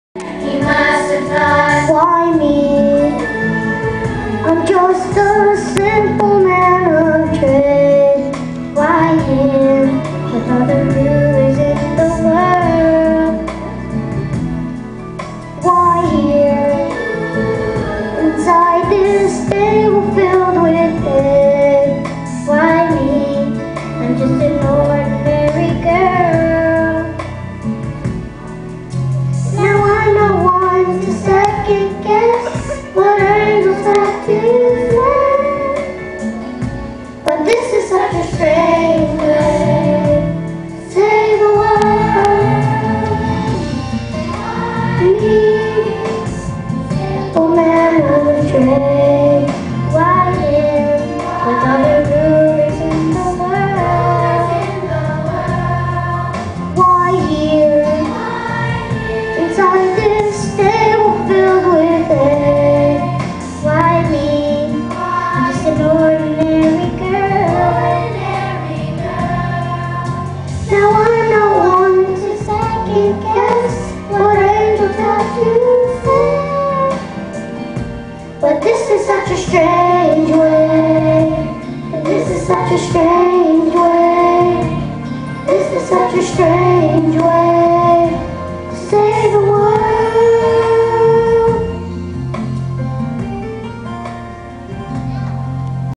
Dakota and Dayna Singing
A clip of our cousin Cody as Joseph singing at this year's Children's Musical at church. He is accompanied by Dana as Mary.